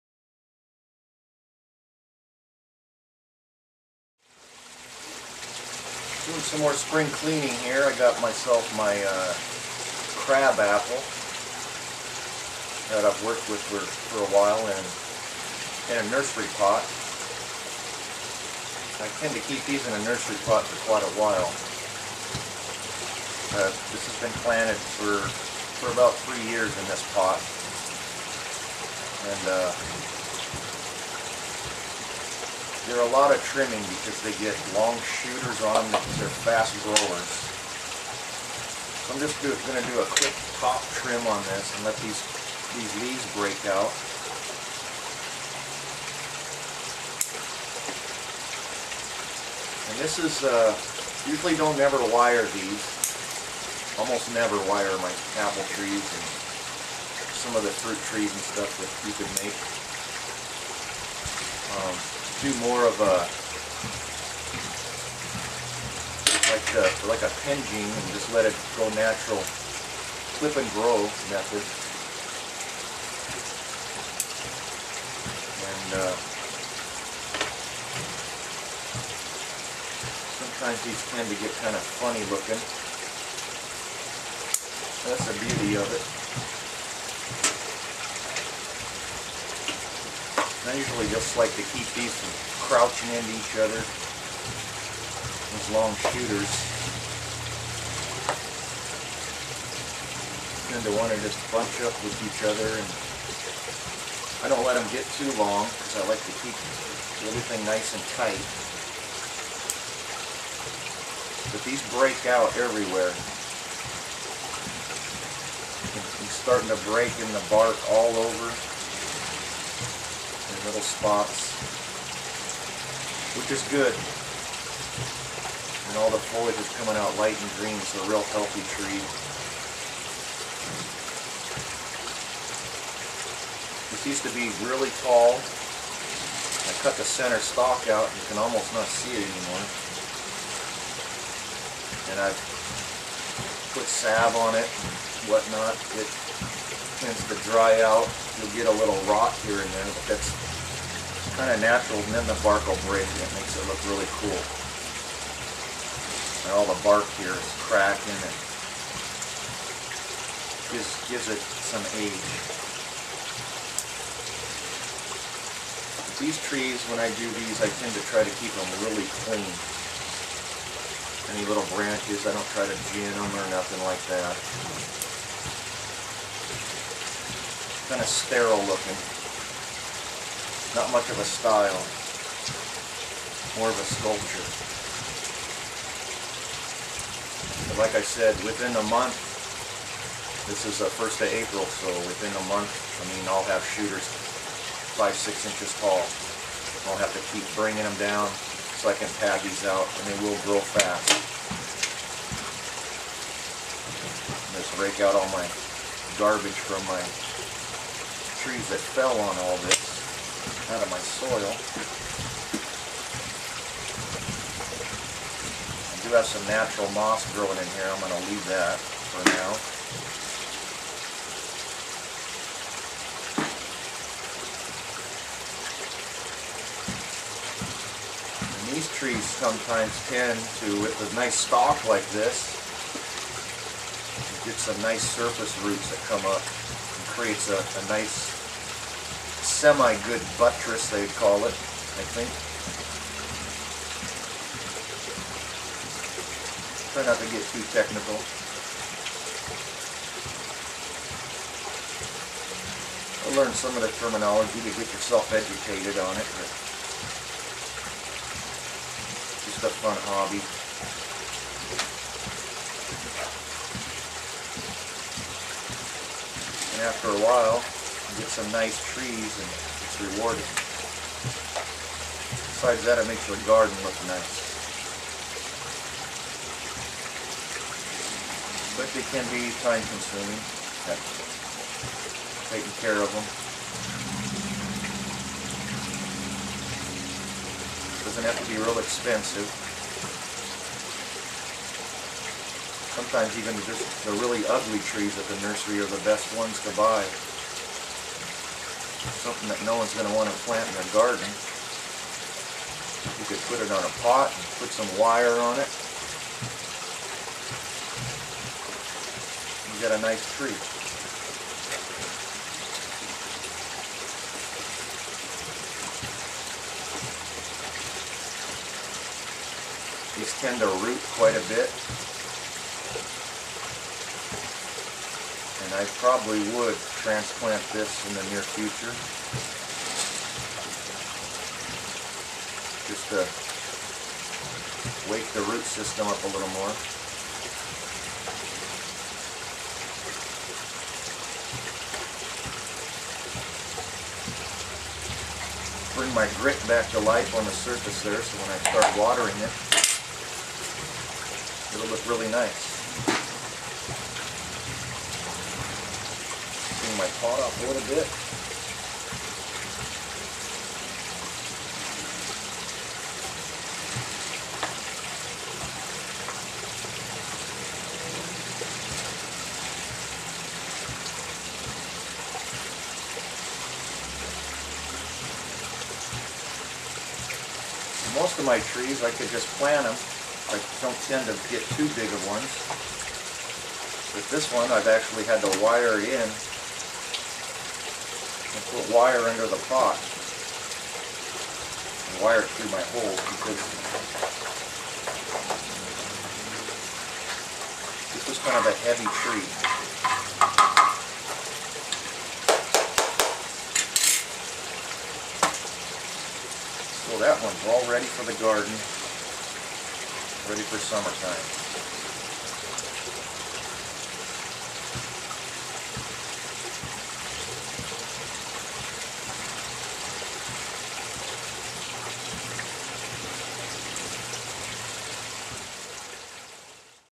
Bonsai Crabapple spring cleaning
a work in progress.  cleaning my apple tree for spring.  let me know what you think.